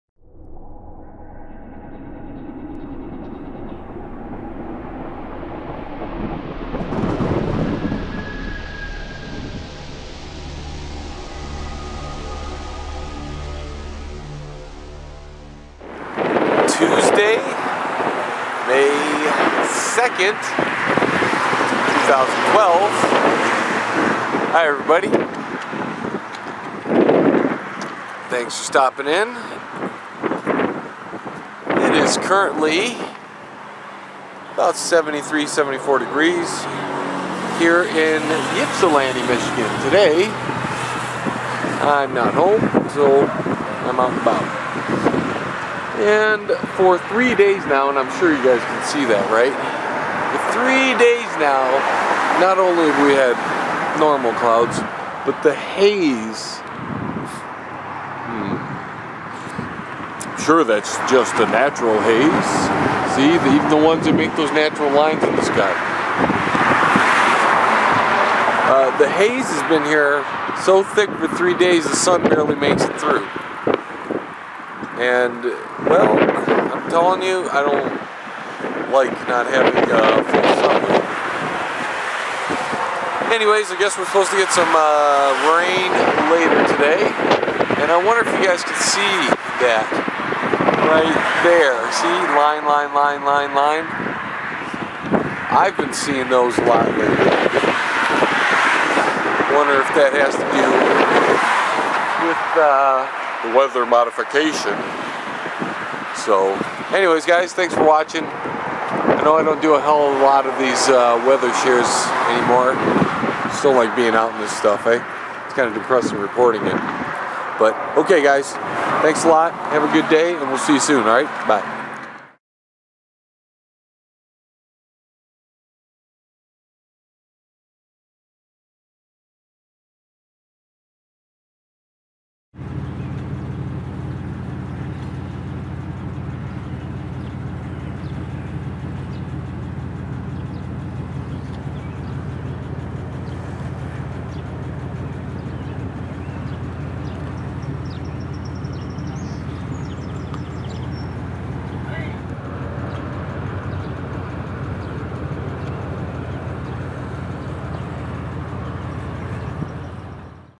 WEATHERSHARE 5/2/2012 Ypsilanti MI - 3day haze/HAARP lines and a orb at 2:33
filmed May 2 2012 in Ypsilanti MI